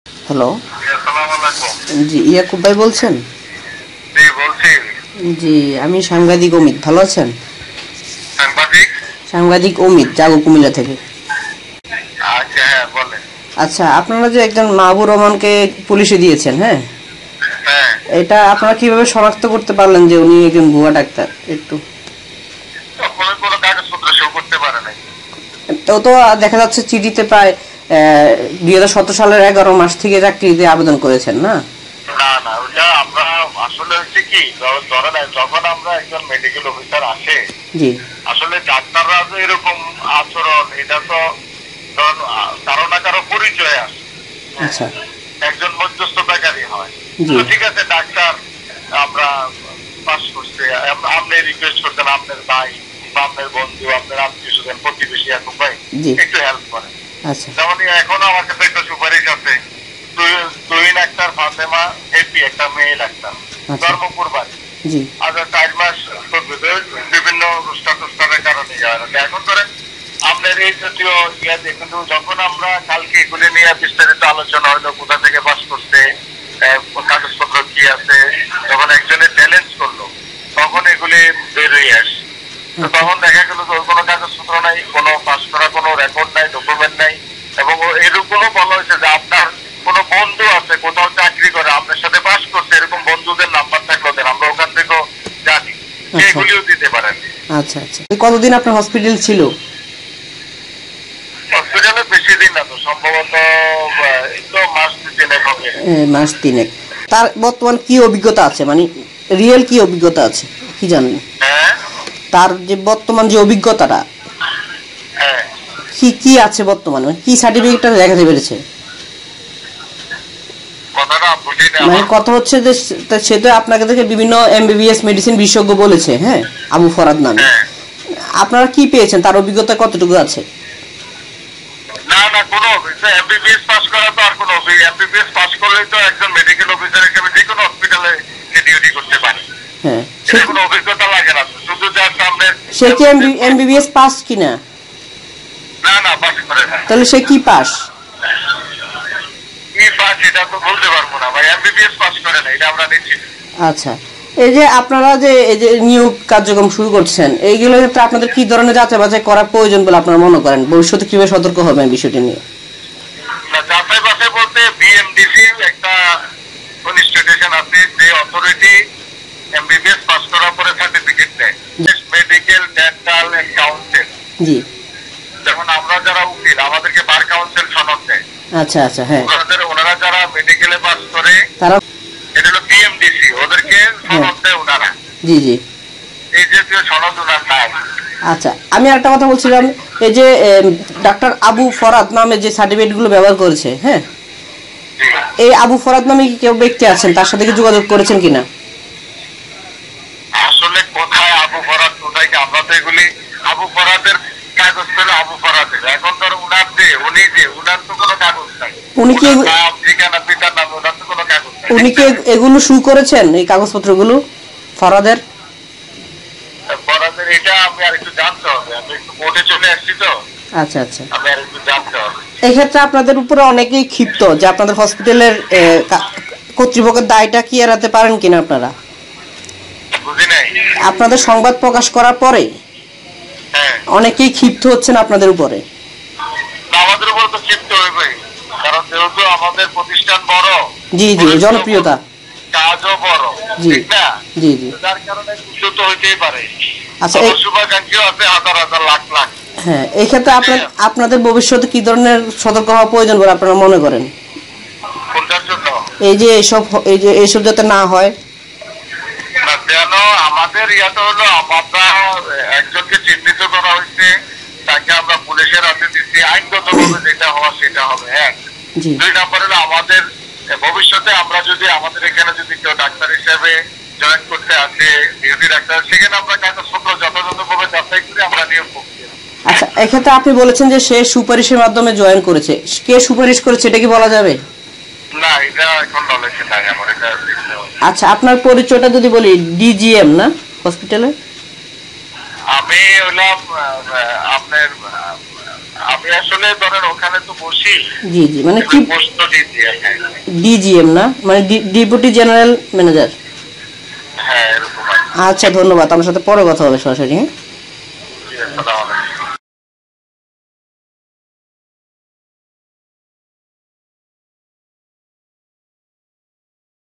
সুপারিশের মাধ্যমে টাওয়ার হাসপাতালে নিয়োগ হয় সেই ভুয়া ডাক্তার! ( অডিও)
সুপারিশের মাধ্যমে টাওয়ার হাসপাতালে নিয়োগ হয় সেই ভুয়া ডাক্তার! ( অডিও) বিস্তারিত দেখতে ভিজিট করুন: jagocomilla.com